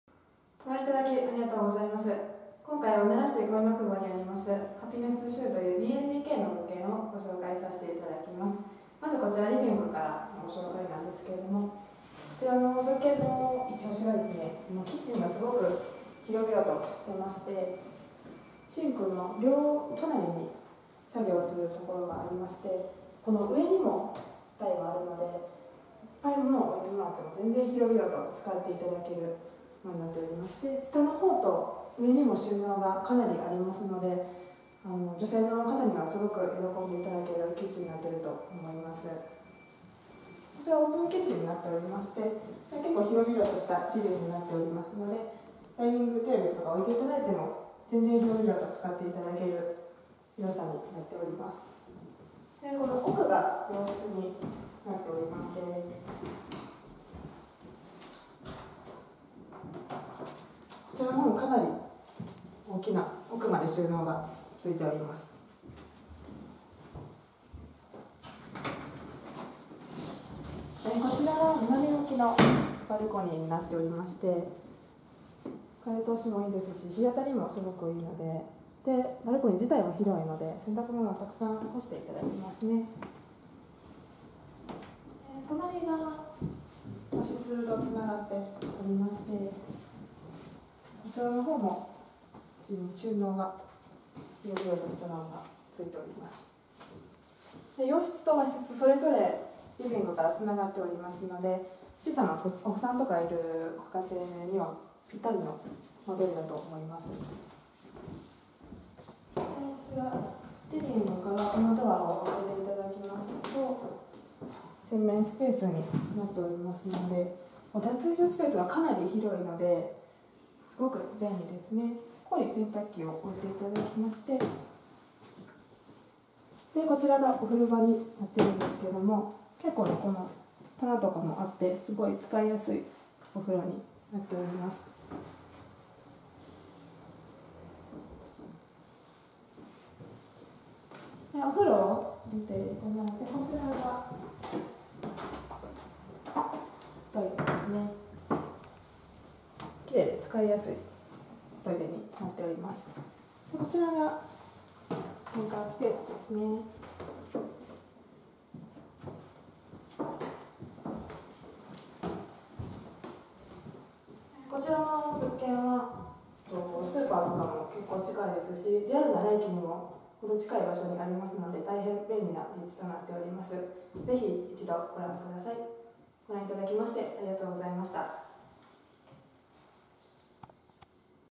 【ルームツアー】ハピネス秀｜奈良市奈良駅賃貸｜賃貸のマサキ｜Japanese Room Tour｜000314
000314
奈良県奈良市恋の窪１丁目4-15にあります評判の良い間取りの2LDKマンション【ハピネス秀】の物件紹介動画です。